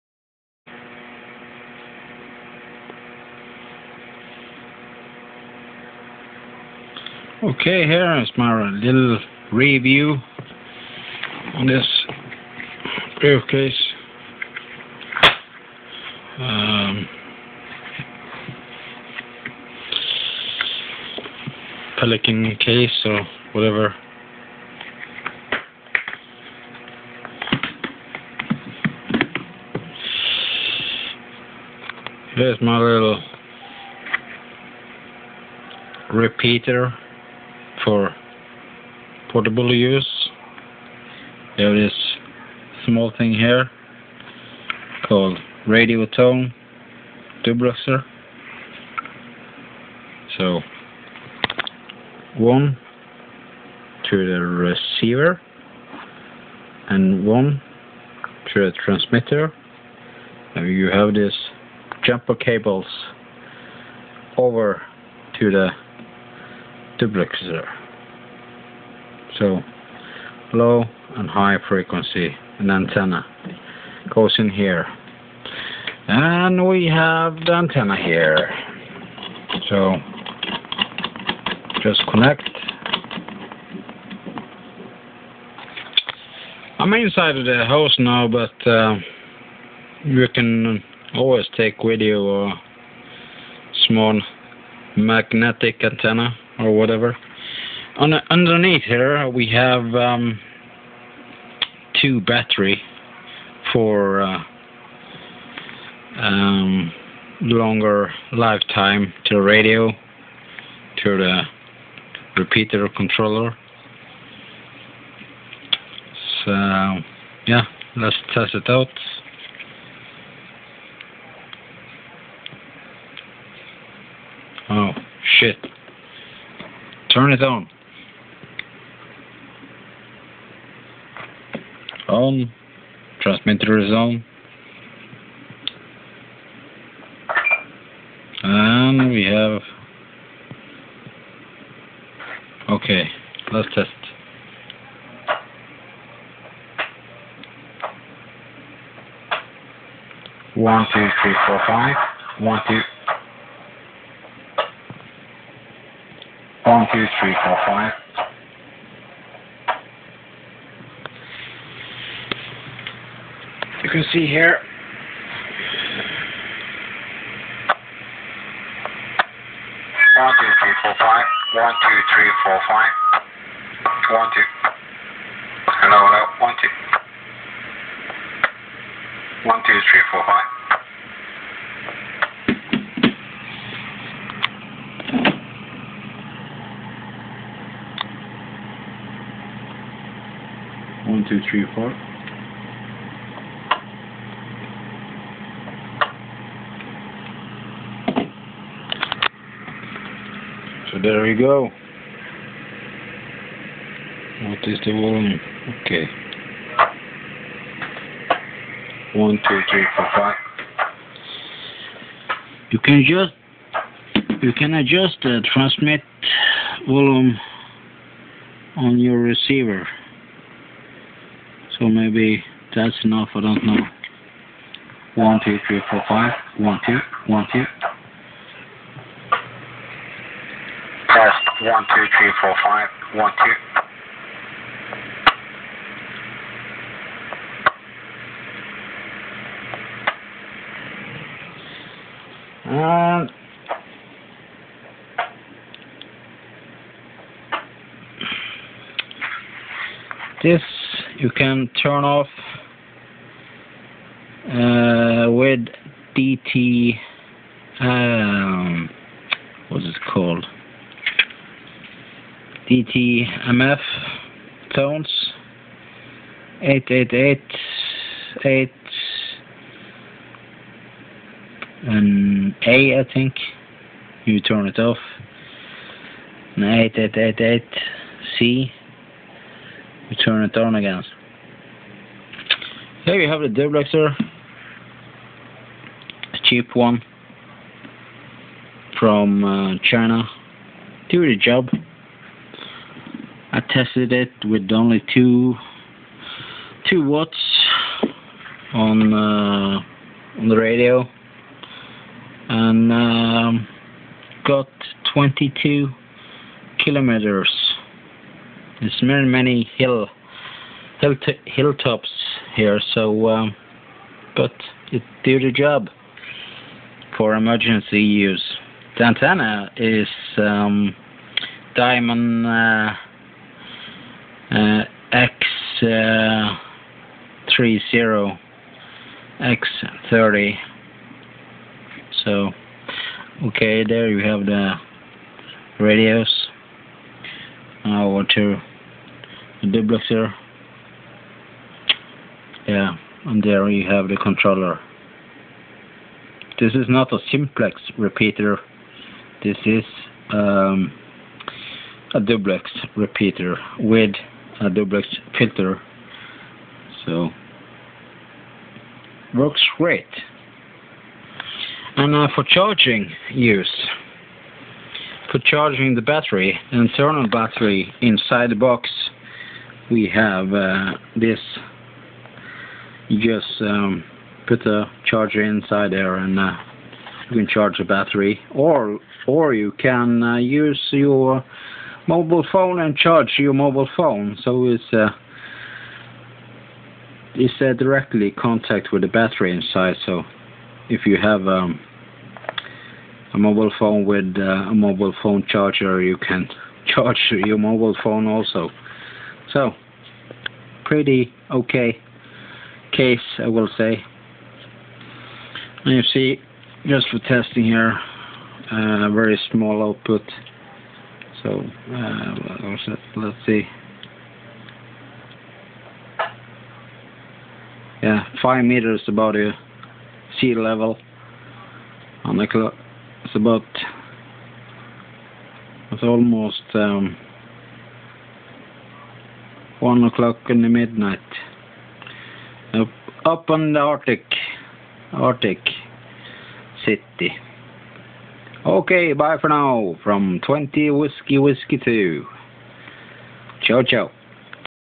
RadioTone VHF Repeater with duplexfilter Part 2 update
Made me a emergency or for field use.. portable repeater in a pelicancase
Great to take with you when you need more range. Under the radio and duplexer it has 2 12volt 7,2 amp batterys so i can connect to the radio and repeatercontroller "RadioTone" and on the side of the case it has 12 volt input for charging the battery and 12 volt for charging a handradio or mobilephone +++

This was only a test and is not in use but can be if in need as a emergency +++
Used the Daimond X30 antenna and on low power on the radio "1-2watt" in this hilltop area it did the job good in the test, about 23km, so if i had place it on a high ground "hilltop and full power on the radio that is 5 watt it will do very good
You have also 2m "vhf" amplifier cheap ones on the nett for 80-100USD that give the repeater 50-100watt after the duplexfilter so..

So in the end, you can make a very cheap proff and stabile repeater